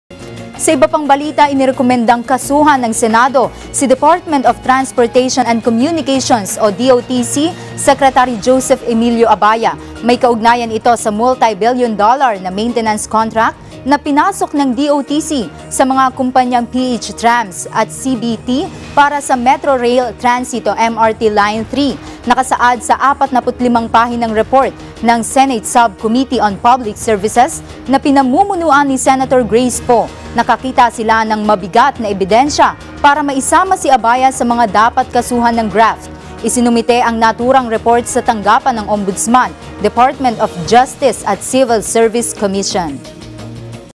Senate sub-committee recommends filing of graft charges against DOTC Secretary Abaya
According to the Senate sub-committee on public services led by Senator Grace Poe, Department of Transportation and Communications Secretary Joseph Emilio Abaya should be charged with graft in connection with the multi-billion dollar maintenance contract entered into by said Department.